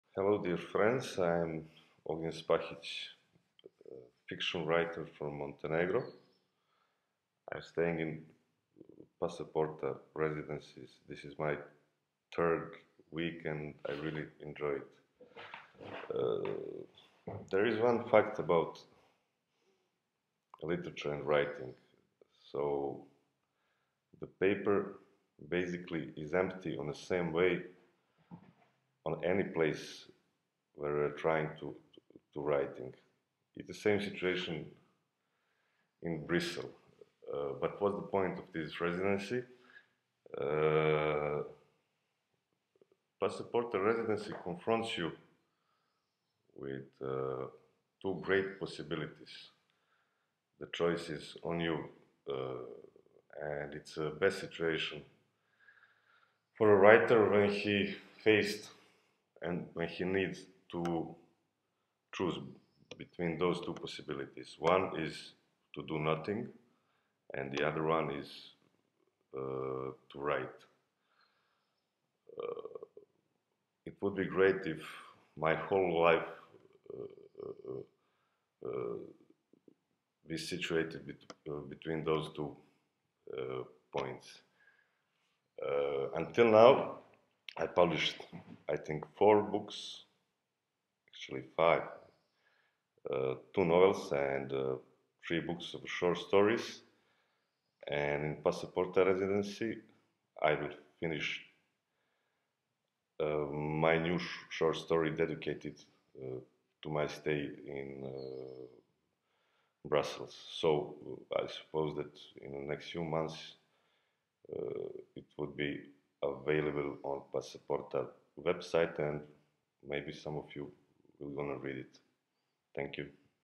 writer in residence Ognjen Spahic
"The paper is empty in the same way at any place"

Ognjen Spahic, Montenegrin author and winner of the European Union Prize for Literature 2014, is staying at the Passa Porta writer's flat at the Place du Vieux Marché aux Grains in Brussels. During his stay, he was working on a new novel.